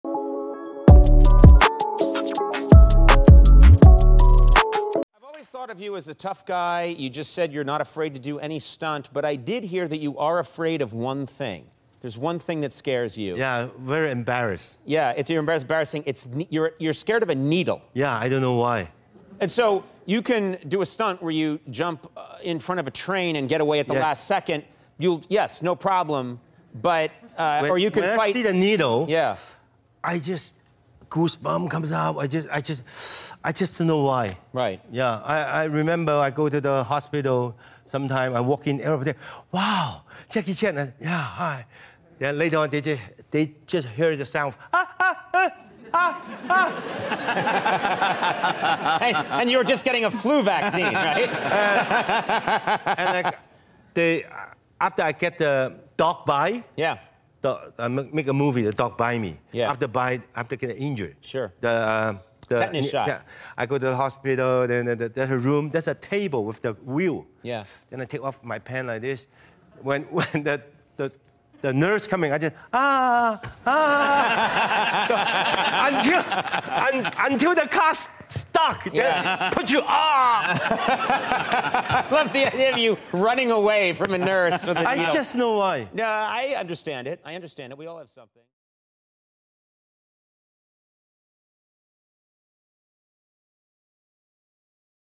JACKIE CHAN NÃO TEM MEDO DE NADA, EXCETO UMA COISA... | ENTREVISTA LEGENDADA
Na entrevista legendada, Jackie Chan comenta um e possivelmente seu único medo no mundo!